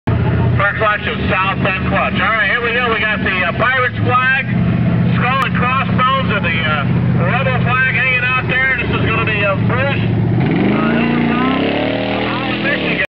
Bits N Pieces, sodbusters, 2008, spring, leaving
Adam leaving the pit, and Bruce in the creeper at the line.